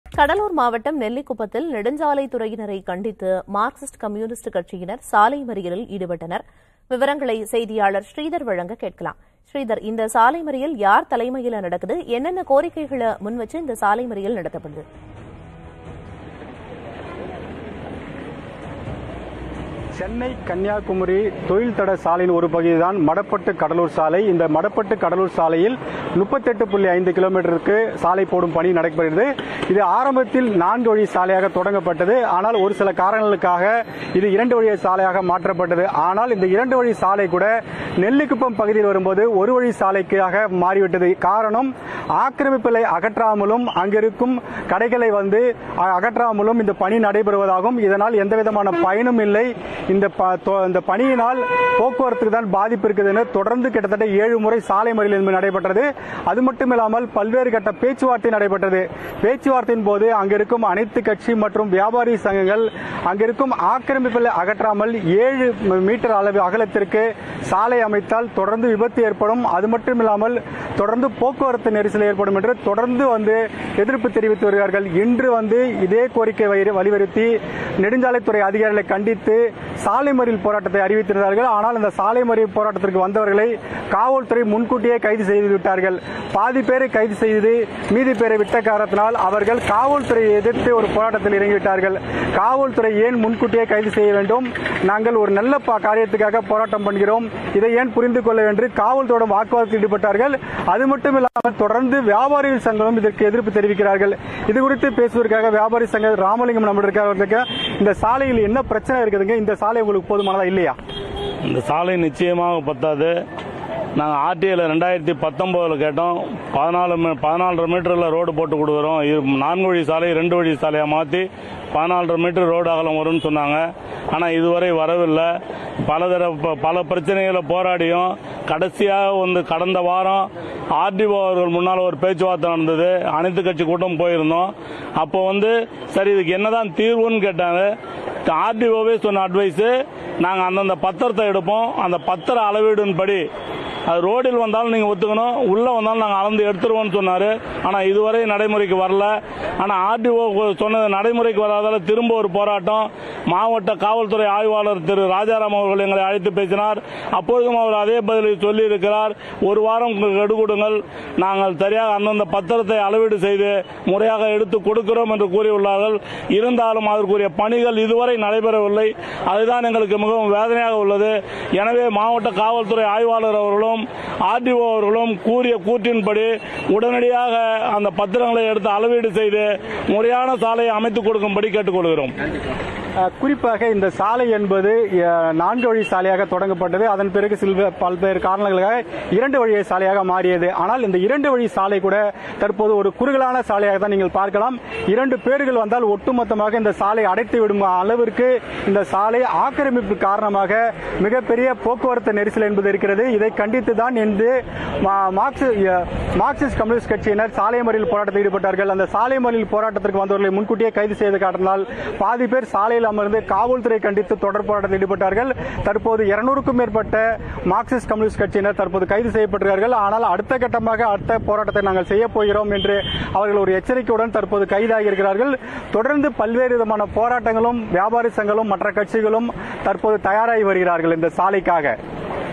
கடலூர் மாவட்டம் நெல்லிக்குப்பத்தில் நெடுஞ்சாலைத்துறையினரை கண்டித்து CPI(M) சாலை மறியல்
கடலூர் மாவட்டம் நெல்லிக்குப்பத்தில் நெடுஞ்சாலைத்துறையினரை கண்டித்து CPIM சாலை மறியல்